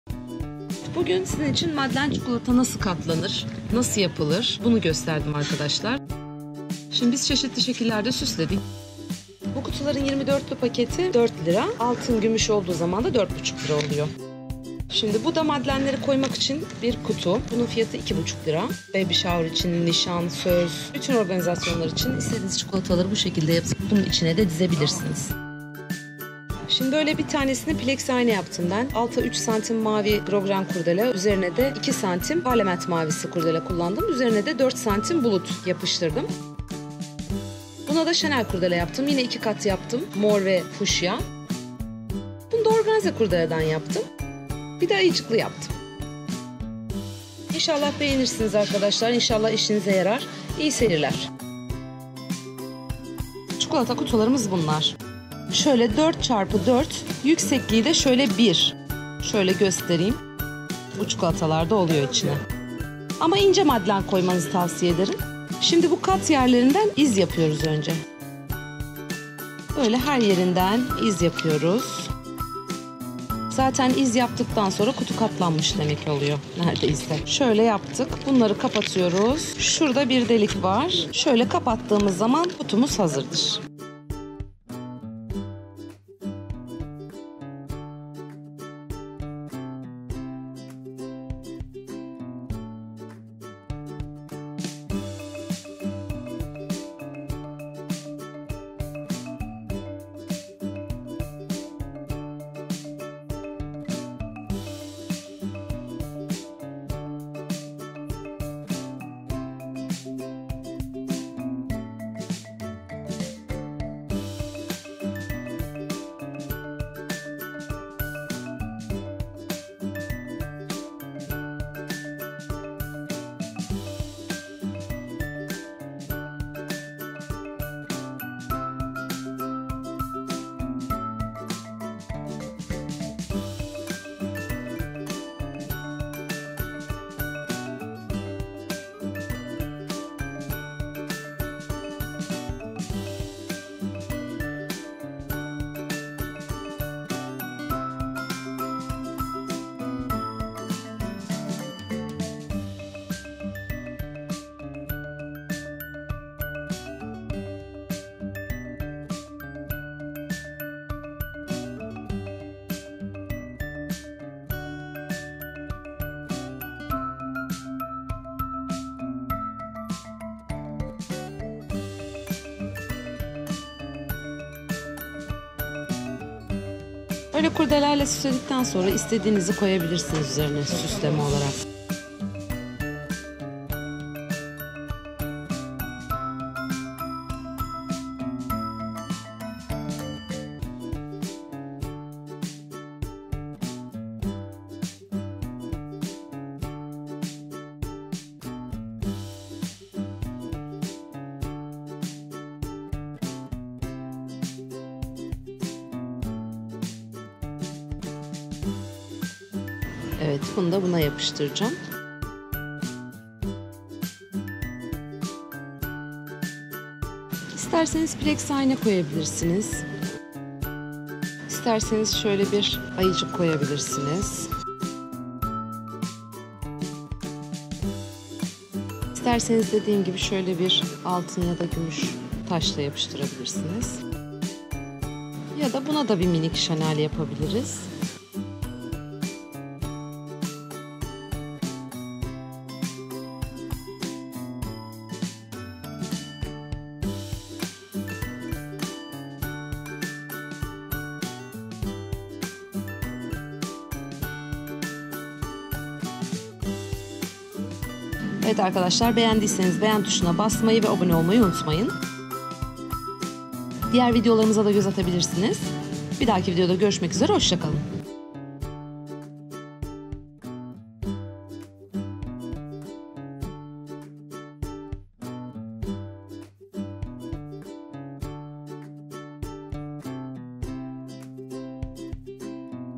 Madlen Çikolata Kutusu Yapımı - DIY #41
Merhaba,
Bu videoda söz, nişan, kız isteme gibi özel günlerinizde kullanabileceğiniz çikolata kutusu yapımını çektik.
İyi Seyirler.